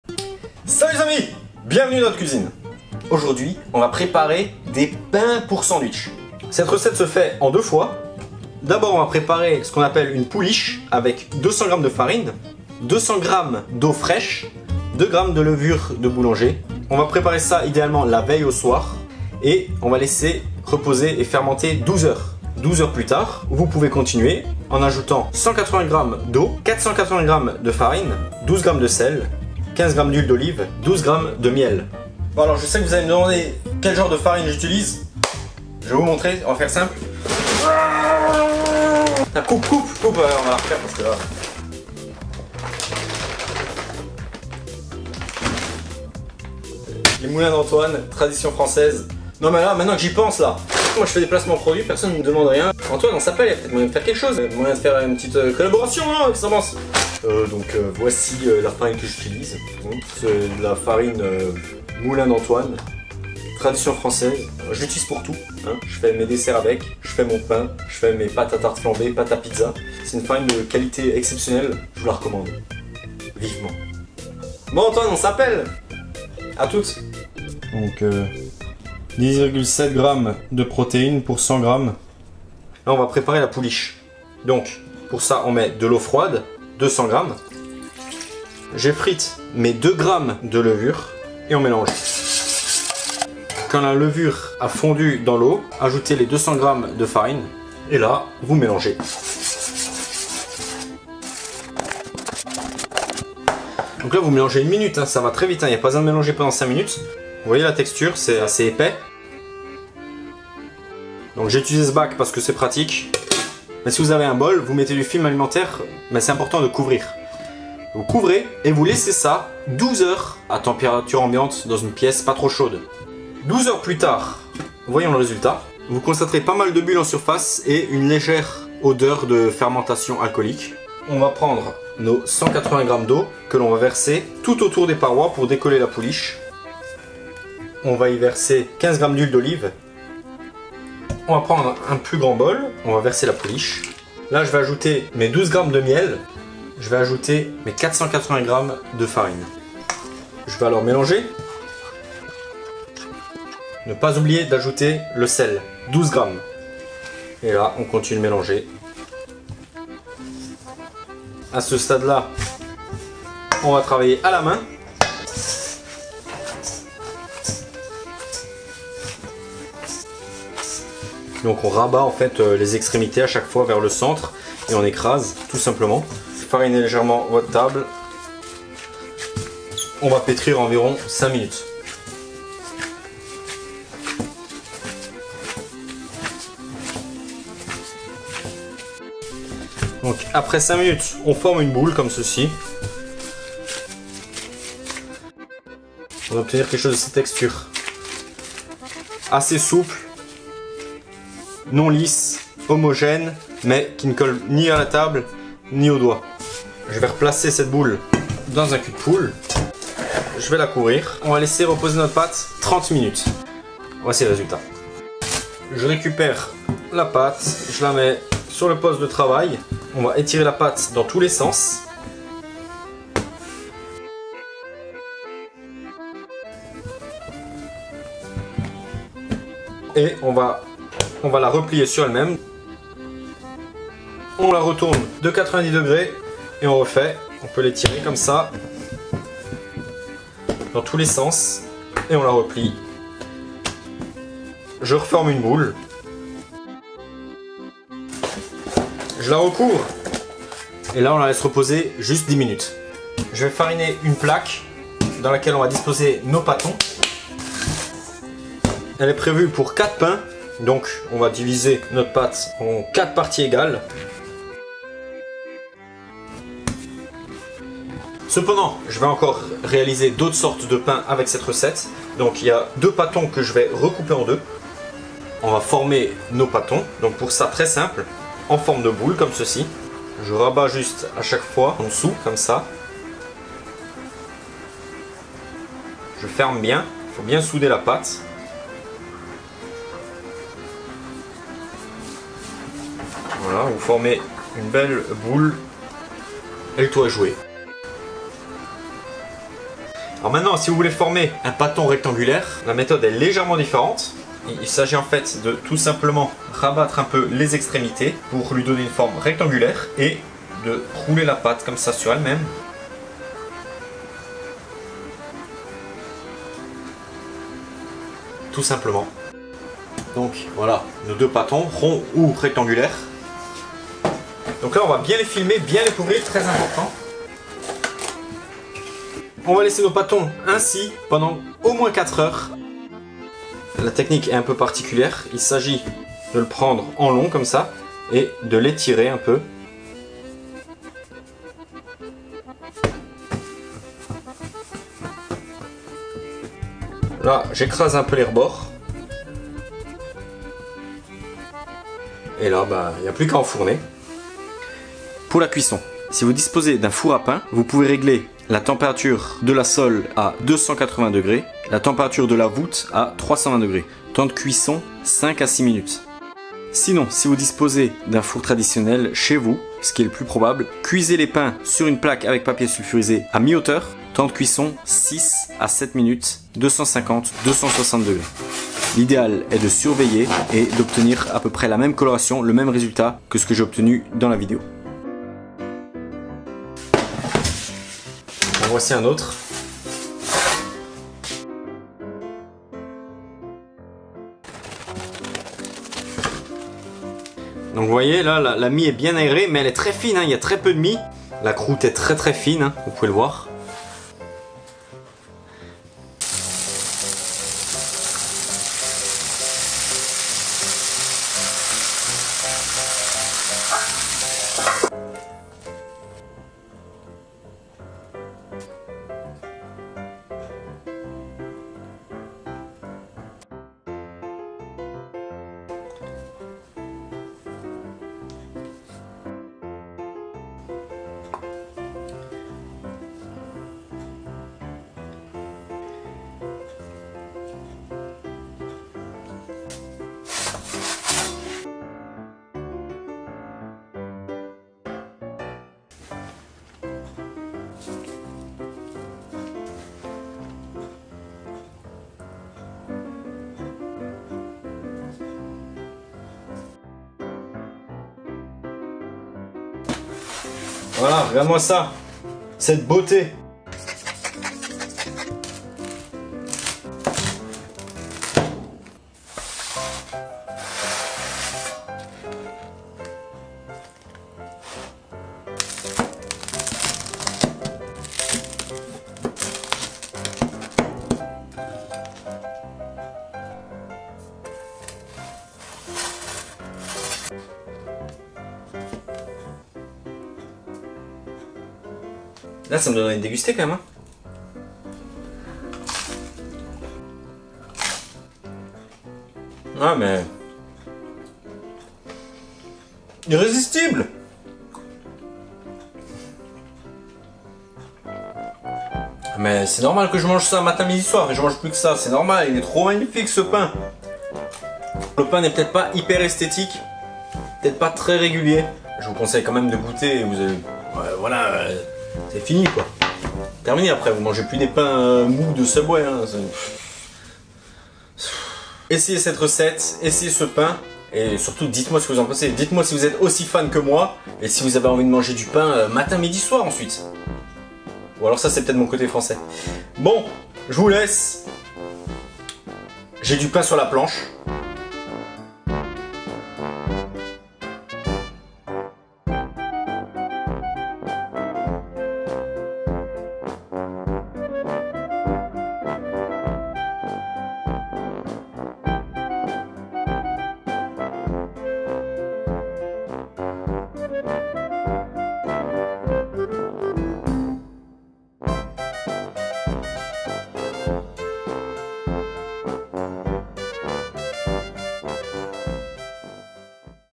跟著法國人做烘焙》我最愛的三明治麵包食譜 (My FAVORITE Sandwiches breads Recipe)
嗨我的朋友們！

今天我要跟你們分享史上最讚的三明治麵包食譜！自從找到這份食譜後，我幾乎每一餐都在吃這個麵包或做三明治XD 我真的超愛它！
所以我想說一定要跟你們分享一下，希望你們會跟我一樣喜歡它！

材料（可做4份）
第一階段
-冰水200克
-麵粉200克
-烘焙酵母2克
第二階段（12小時後）
-常溫水180克
-麵粉480克
-鹽12克
-橄欖油15克
-蜂蜜12克

Today I’m sharing with you my favorite sandwiches breads recipe!
Since I found this recipe, I didn’t stop eating bread and sandwiches at every meal! I totally fell in love!
Then I felt I should definitely share it to you!
Hope you’ll enjoy as I do!

For 4 breads :
1st step
-200g cold water
-200g flour
-2g bakers yeast

2nd step (12hours later)
-180g water
-480g flour
-12g salt
-15g olive oil
-12g honey

FRANÇAIS
Salut mes amis !

Aujourd'hui je partage avec vous ma recette de pains sandwichs préférée!
Depuis que j'ai trouvé cette recette, je n'ai pas arrêté de manger du pain et des sandwichs à tous les repas ! Je suis totalement tombé amoureux !
Alors je me suis dit que je devais absolument la partager avec vous !
J'espère que vous apprécierez autant que moi !

Pour 4 pains :
1ère étape
-200g d'eau froide
-200g de farine
-2g de levure de boulangerie

2ème étape (12 heures plus tard)
-180 g d'eau
-480 g de farine
-12 g de sel
-15 g d'huile d'olive
-12 g de miel